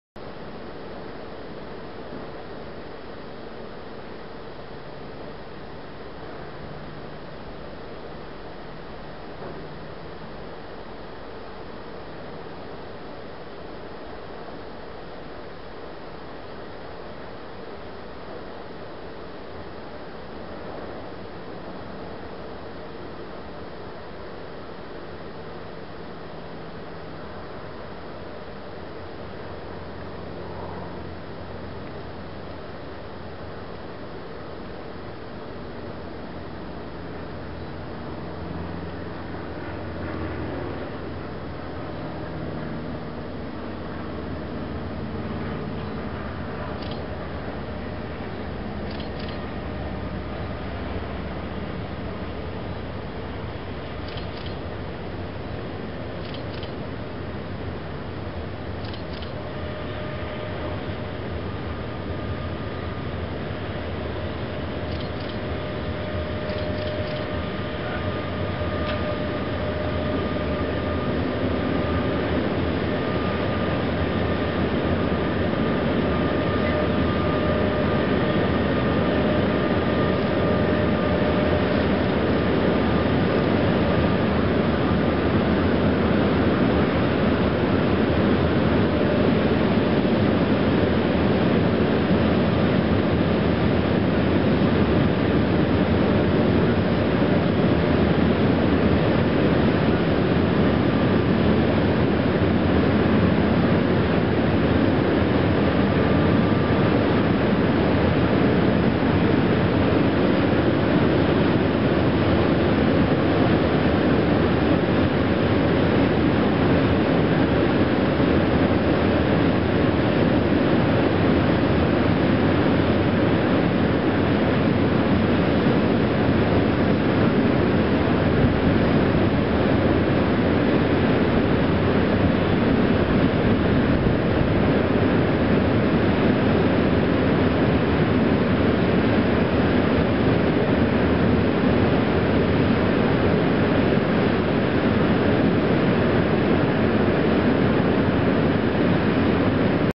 E186 250-7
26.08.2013, E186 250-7 prowadzi próżny pociąg ze Świnoujścia do Płocka szlakiem Poznań Piątkowo - Koziegłowy.